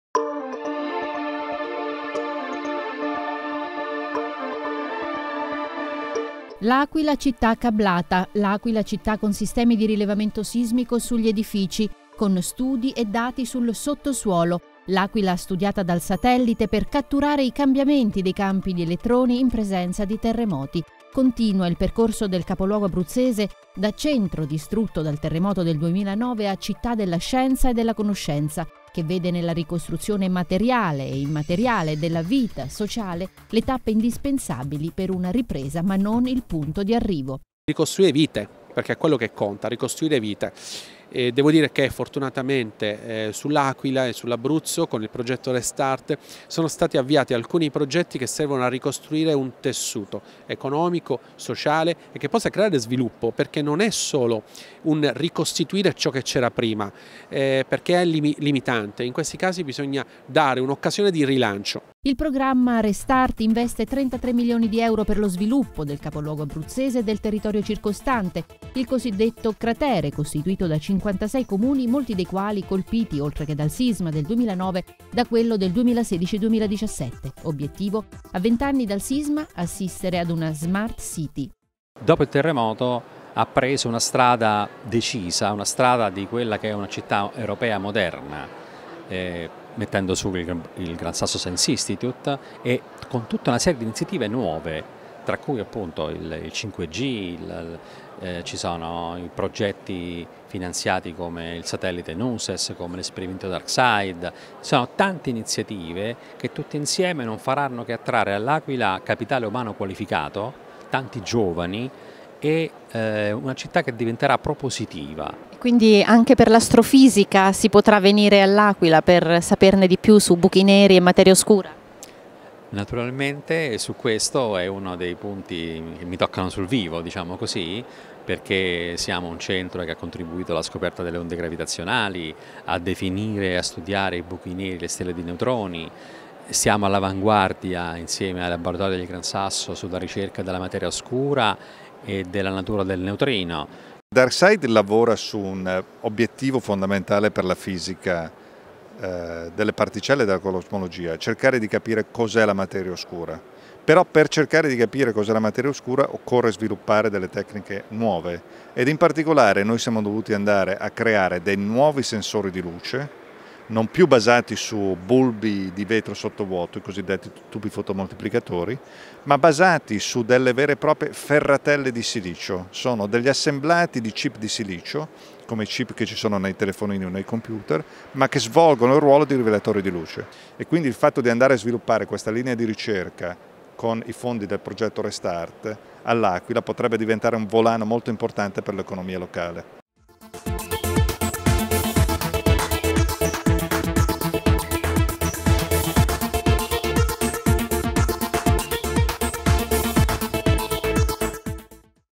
Restart Abruzzo: Trasformare un territorio dopo il sisma in una smart area
Convegno "Tecnologie avanzate e sviluppo. Trasformare un territorio dopo il sisma in una smart area"
Roma, 14 maggio 2019, ForumPA 2019

Servizio di Claudia Cichetti
Riprese di Ubimedia e Giovanni Lacetera
Post-produzione Giovanni Lacetera